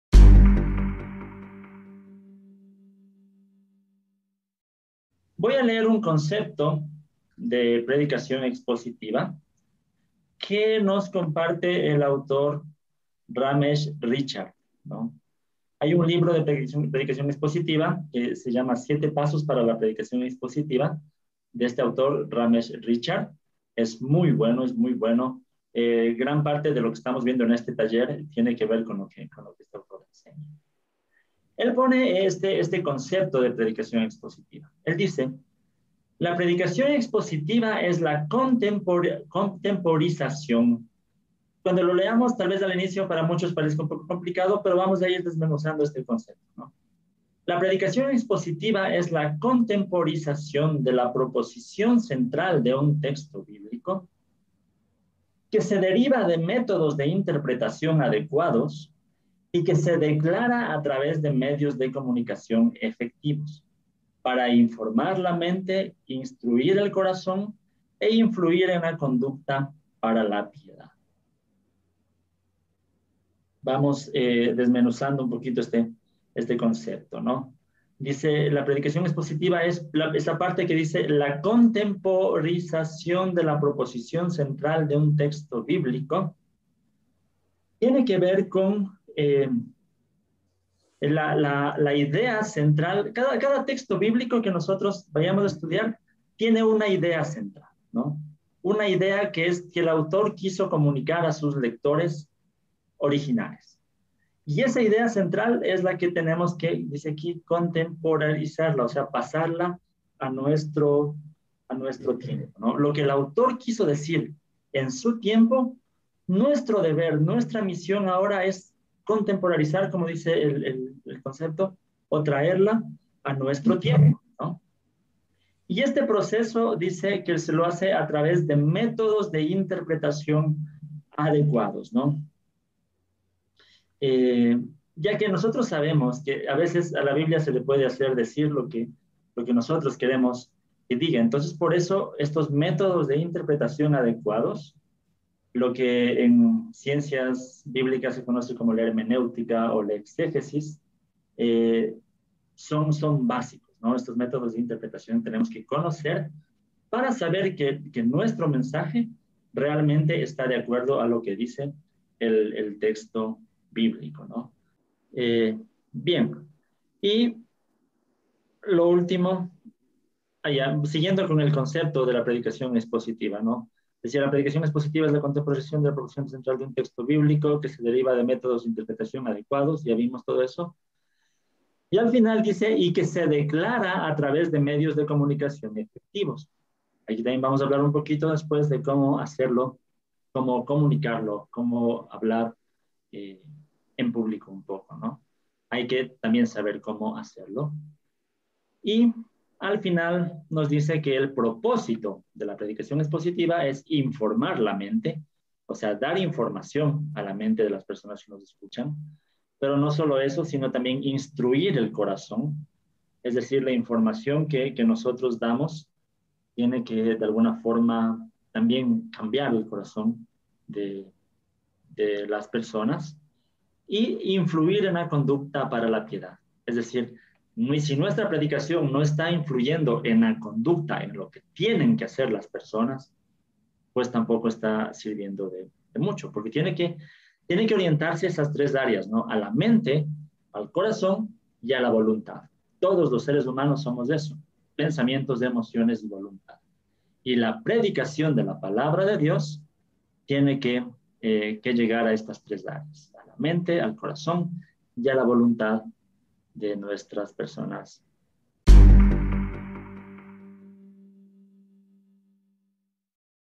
¿Qué es la PREDICACIÓN EXPOSITIVA? - Canal Cristiano - TodoTeos
Extracto del Taller de Predicación Expositiva Del Texto al Mensaje.
Pueden mirar el Taller completo desde la sección de Miembros del Canal en el siguiente enlace